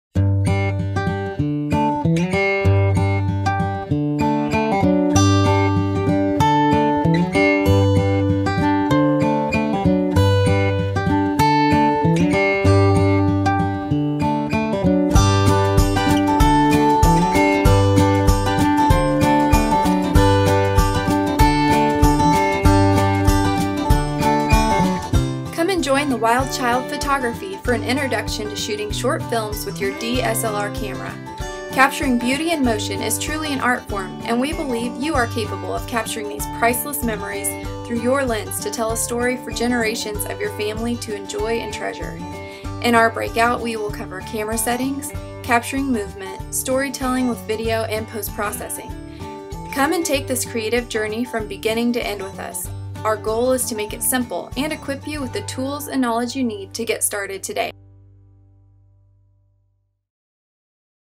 Photography in Motion: The Photographer's Guide to Everyday Video
This Breakout will teach you everything that you need to know in order to incorporate video into your work. Amy Cyphers and Kim Bear of the Wild Child Photography will guide you on a journey to find motion and emotion in your work, allowing you to make everyday videos with your camera. Along the way you’ll learn how to direct creatively, create a vision using storyboarding, and use Premiere to edit your videos. At the end of the Breakout you’ll be able to capture your life in motion and start creating your own family videos (or client videos) as well as videos of everyday moments.